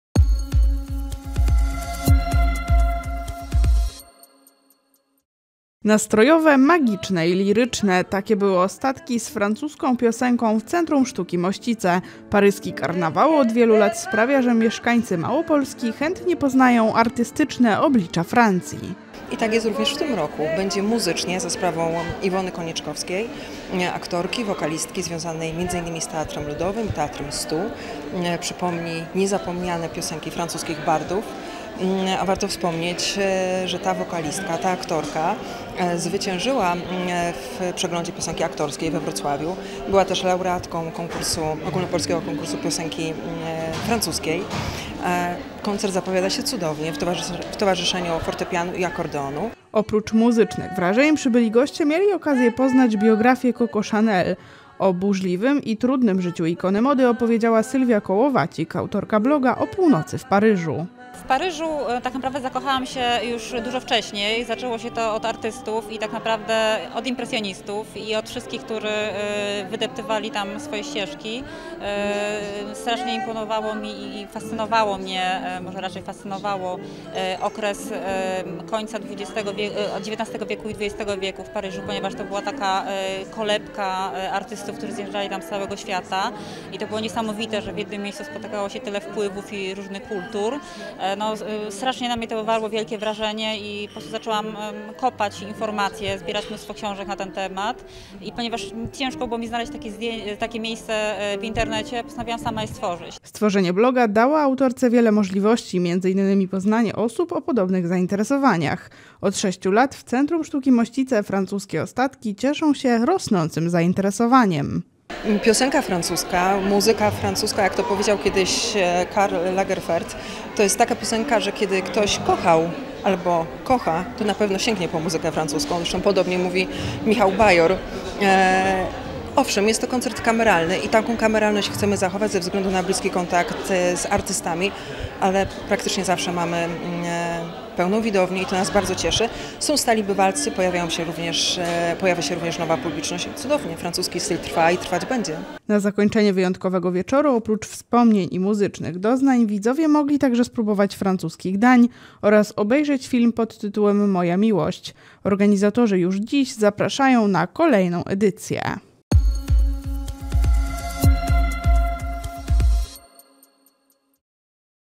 Francuski styl w Centrum Sztuki Mościce
Nastrojowe, magiczne, liryczne... takie były Ostatki z francuską piosenką w Centrum Sztuki Moście. Uczestnicy wzięli udział w wieczorze pełnym atrakcji. Odbyło się spotkanie z Sylwią Kołowacik, autorką bloga O północy w Paryżu, „Paryż, moja miłość” – koncert Iwony Konieczkowskiej z zespołem oraz projekcja filmu „Moja miłość” w reż. Maïwenn.